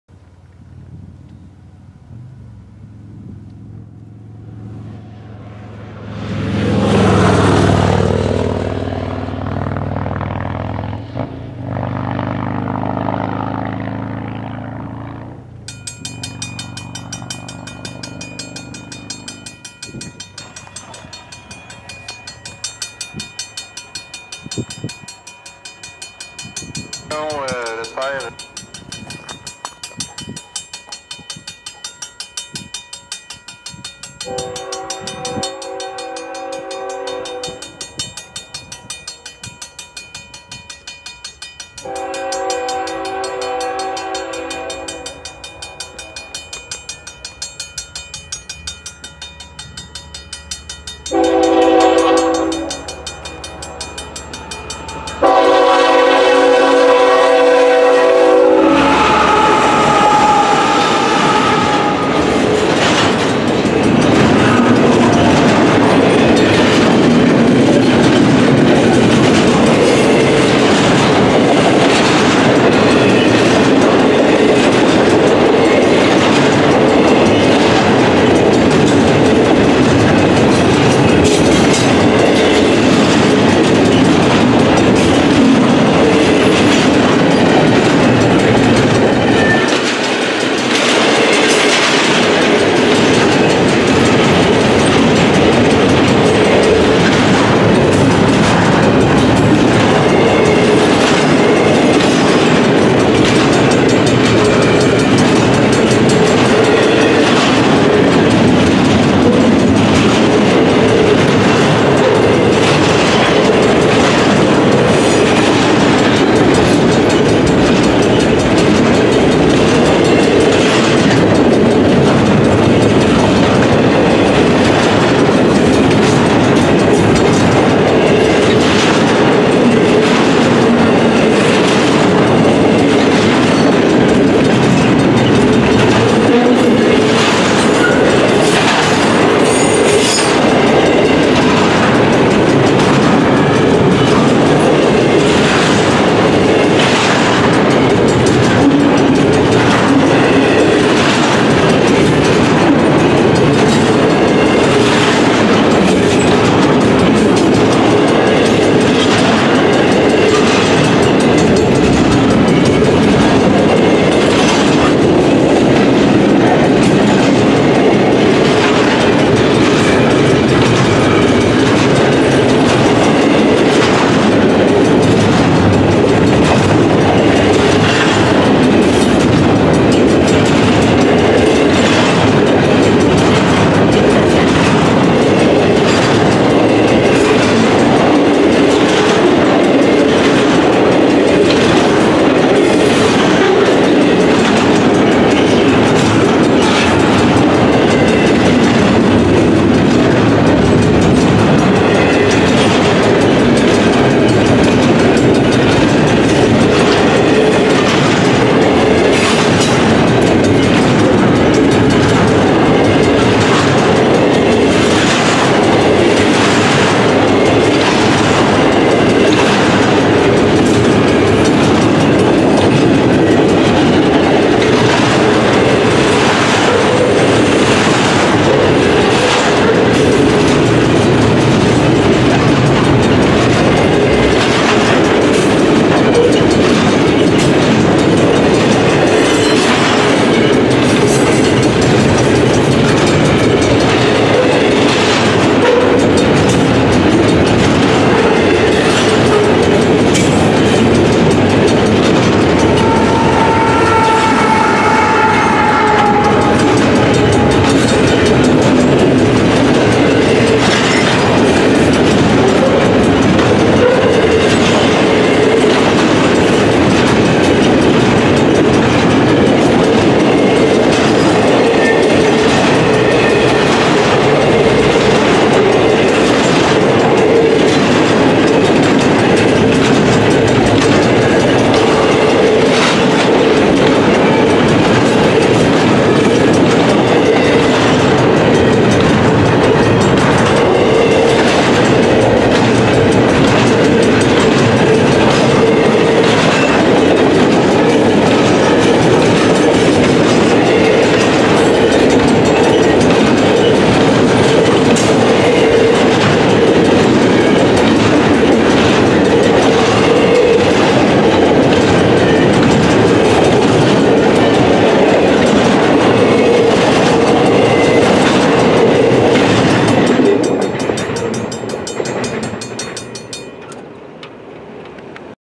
CN Z121 St-André Jct Mile 16.34:CN 3833-3140-3014
CN Z121
28 Avril 2021 14H45
Mile 16.34 Montmagny SUB
CN 3833(ES44AC)
CN 3140(ET44AC)
CN 3014(ET44AC)
xxx Cars